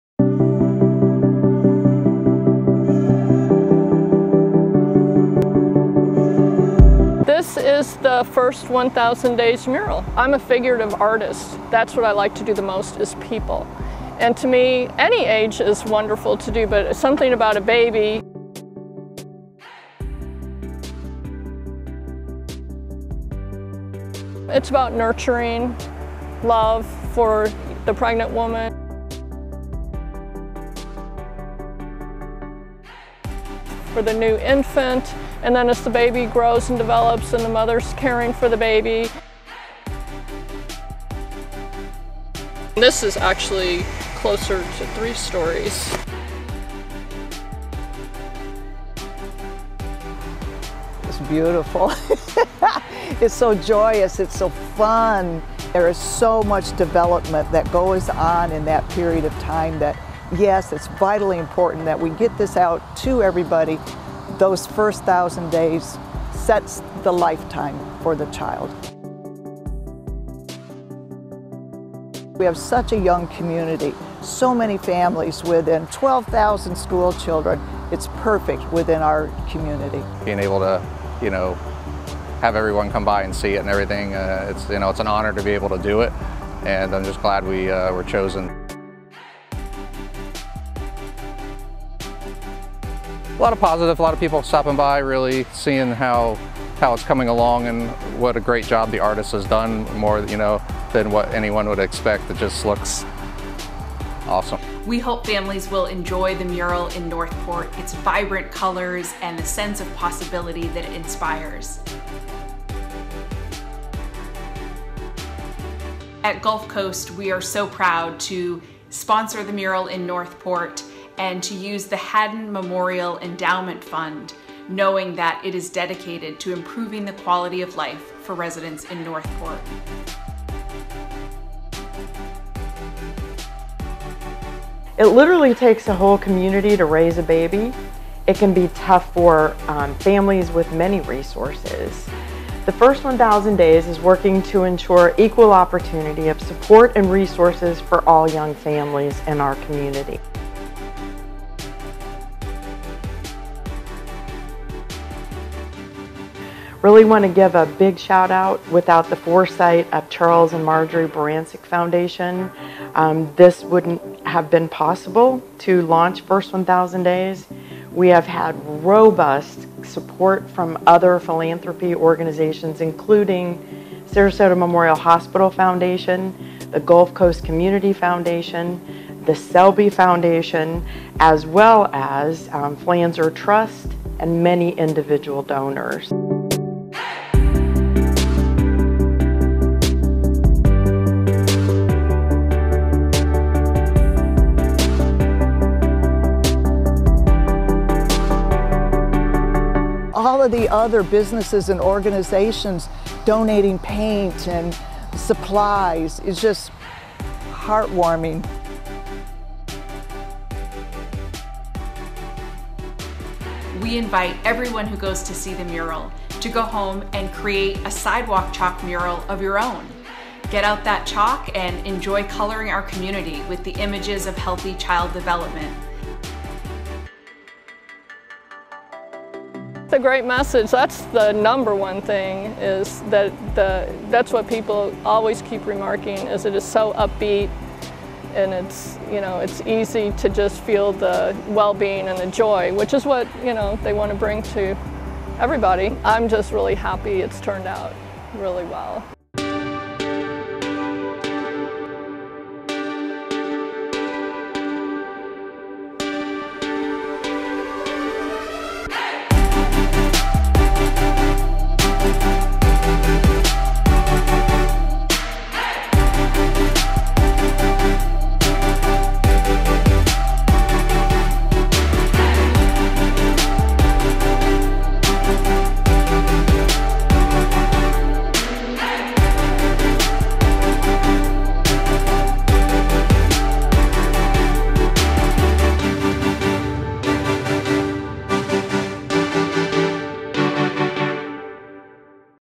First 1000 Days Mural in the City of North Port
A new mural in North Port highlighting a great message and cause. First 1,000 Days Sarasota County is a collaborative effort to give our most vulnerable neighbors the support they need. By pulling together our partners’ resources, this initiative will provide better access to the prenatal, newborn, and early childhood development care that gives babies and families a crucial opportunity to thrive.